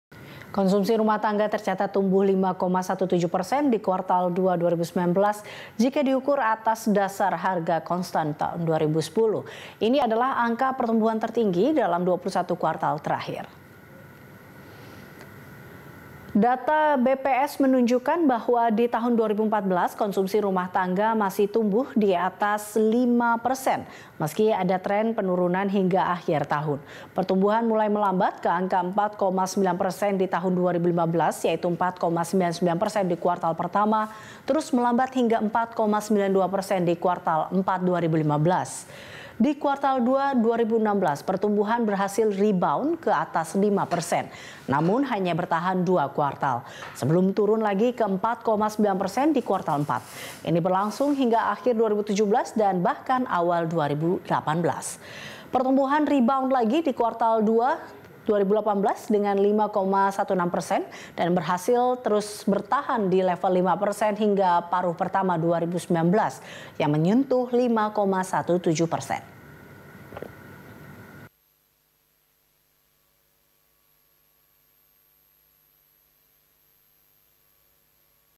Capai yang Tertinggi, Konsumsi Rumah Tangga Tumbuh 5,17%
Konsumsi rumah tangga tercatat tumbuh 5,17% di kuartal dua 2019 jika diukur atas dasar harga konstan 2010. Ini adalah angka pertumbuhan tertinggi dalam 21 kuartal terakhir.

Facebook.com/BeritaSatu
Youtube.com/BeritaSatu